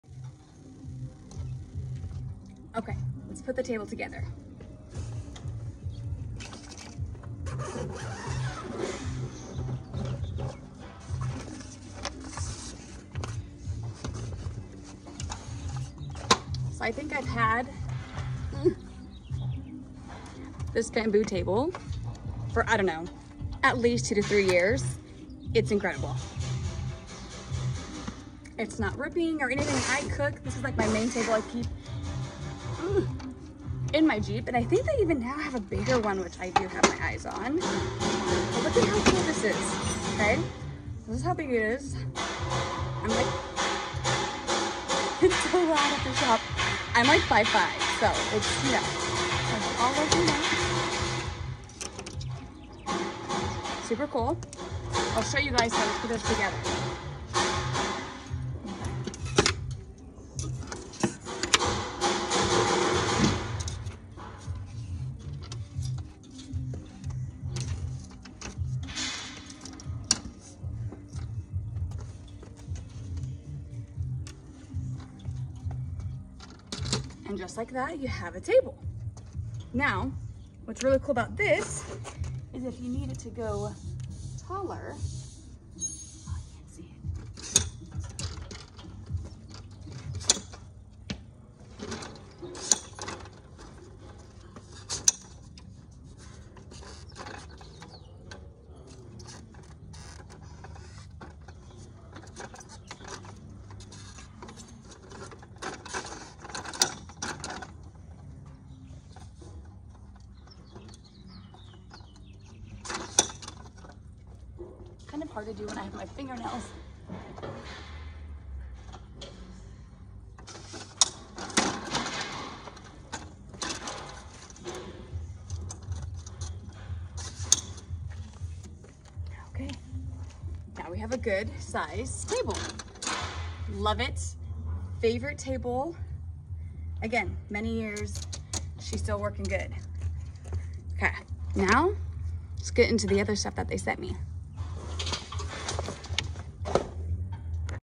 Unboxing my KingCamp Bamboo Folding Table Environmental Camping Table with Adjustable Height
KingCamp Lightweight Stable Folding Camping Table Bamboo Outdoor Folding Tables Adjustable Height Portable Picnic

SPECIFICATION:
Set Size: 25.6x19.6x17.7/20.5/25.6in
Packed Size:25.6x5.5x4.7in
Weight: 9.3lbs
Max load: 176lbs
Fabric: Bamboo
Frame: Aluminum alloy
Carry bag:100% Polyester 600D Oxford
PACKAGE INCLUDES:
1 ×Folding Table
1 × Carry Bag